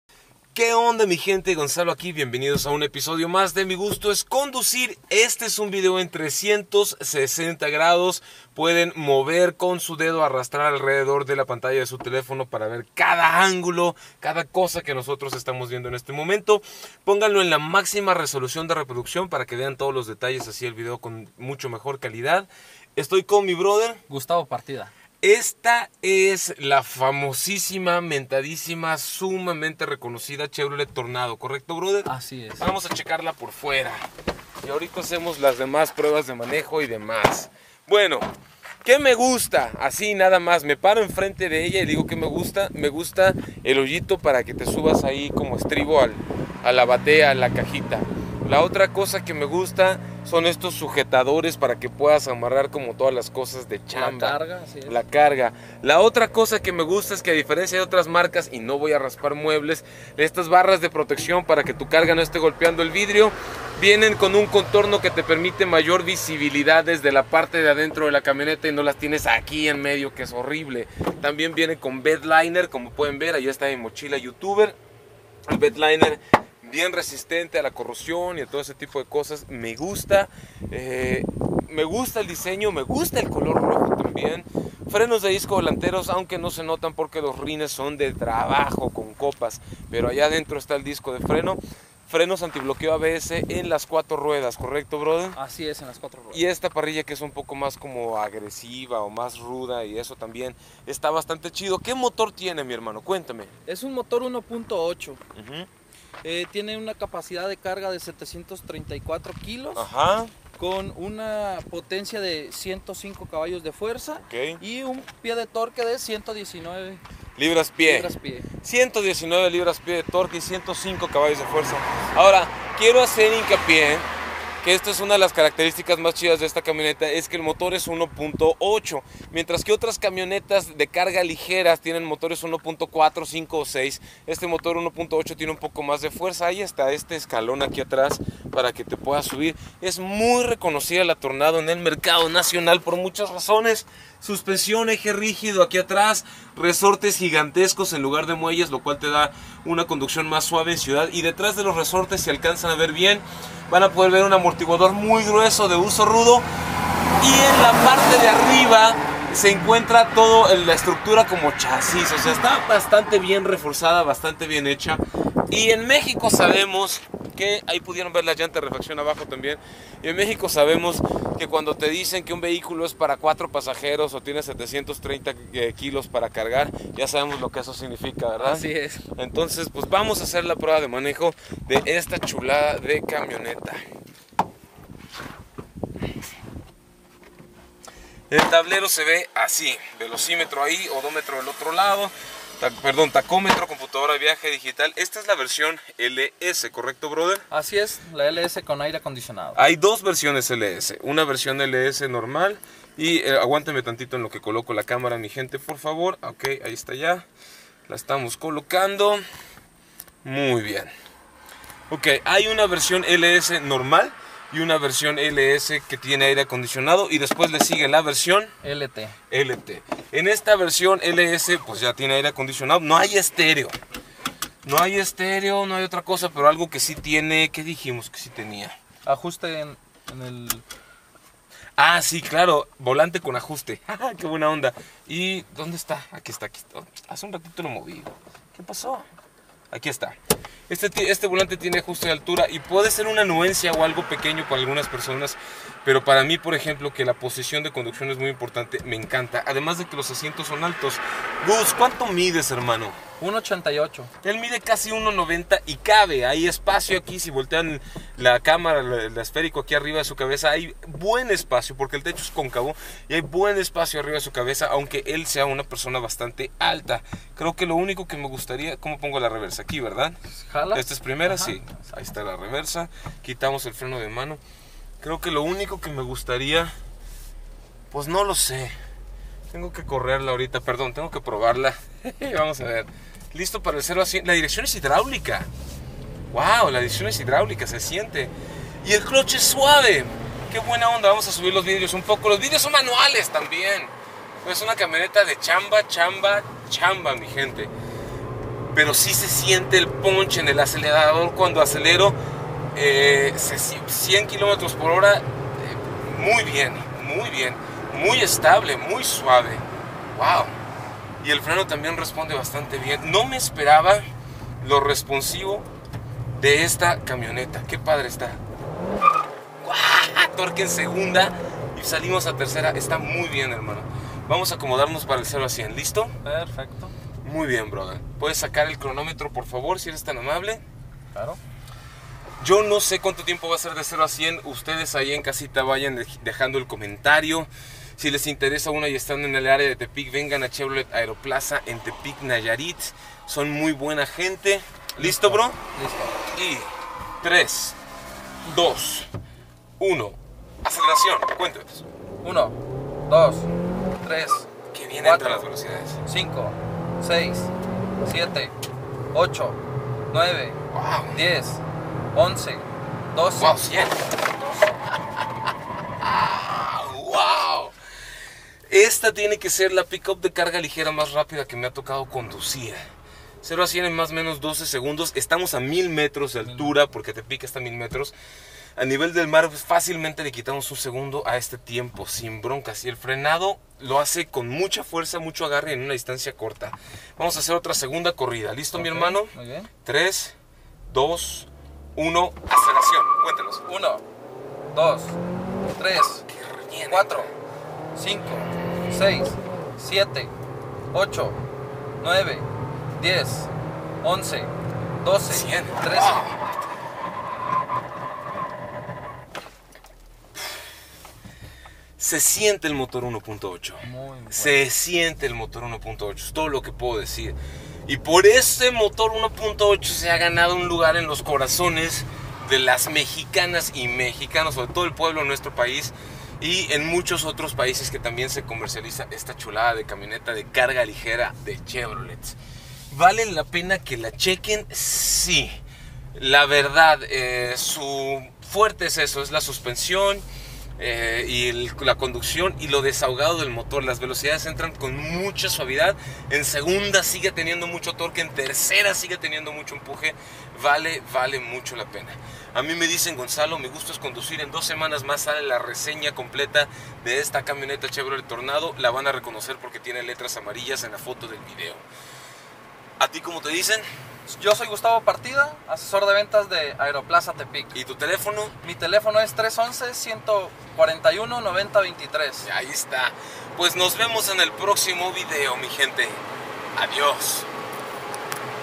🔴 Conoce Chevrolet Tornado ¡Pequeña Gran Camioneta PickUp de Trabajo!
Descuento 20% en seguro de auto si usas mi link personal Conoce Chevrolet Tornado ¡Pequeña Gran Camioneta PickUp de Trabajo!

 *los precios están sujetos a cambios sin previo aviso consulta tu distribuidor más cercano

*Los consumos de combustible son combinados promedio entre ciudad y carretera, pueden variar de acuerdo a tus hábitos y condiciones

Hola que tal mi gente yo soy Gonzalo Magaña y este es una reseña en video  grabado en vivo en 360 grados para que conozcas la Chevrolet Tornado que es una camioneta Pick Up de carga y una camioneta Pick Up de trabajo ligero muy buena muy ruda muy confiable muy aguantadora que se ha ganado una buena reputación en el país además de tener un motor bastante fuerte para su tamaño Así que en este vídeo vas a poder checar cada ángulo y vas a poder ver todo ya que es un video esférico en 360 grados en realidad virtual para que cheques los interiores el equipo con el que viene esta versión de la Chevrolet Tornado también para que la conozcas por fuera cheques el exterior de la Tornado así como echamos un vistazo por debajo en la suspensión trasera un vistazo al compartimiento a la caja de carga a la batea también le hicimos la prueba de manejo la prueba de aceleración 0 a 100 en la prueba de frenado de pánico te vamos a dar las especificaciones de esta camioneta Pick Up chica de 4 cilindros con un motor 1.8 y un gran desempeño te vamos a hablar de la potencia en caballos de fuerza a libras pie de torque inclusive darte algunos datos de consumo de rendimiento de combustible en fin todo lo que necesita saber para que la vayas conociendo mejor es lo que publicamos dentro de una semana más la reseña completa de esta Chevrolet Tornado ya con todas las tomas de belleza el formato de vídeo normal Mientras tanto aquí te dejamos toda la información que necesitas para conocer la Chevrolet Tornado que es una camioneta Pick Up de trabajo o una camioneta Pick up pequeña de carga ligera, en 360 grados esférico para que cheques cada detalle.